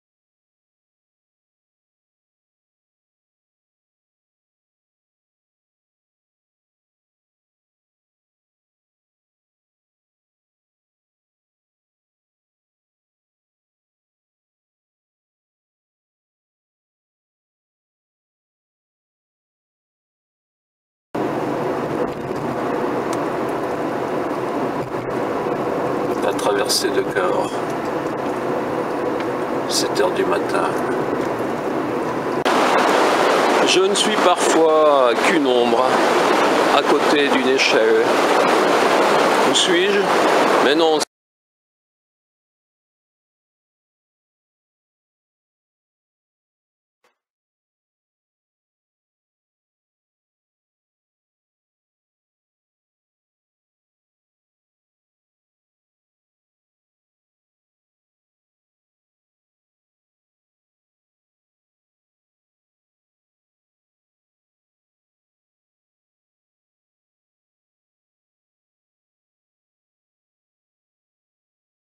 appel téléphonique origine devra peut-être être recherché : 27 octobre 2020 à 15:17 Numéro privé
Un appel téléphonique dont l'origine devra peut-être être recherché : 27 octobre 2020 à 15:17 Numéro privé
Durant 1 minutes 32, où au regard de la situation j'ai donné une information...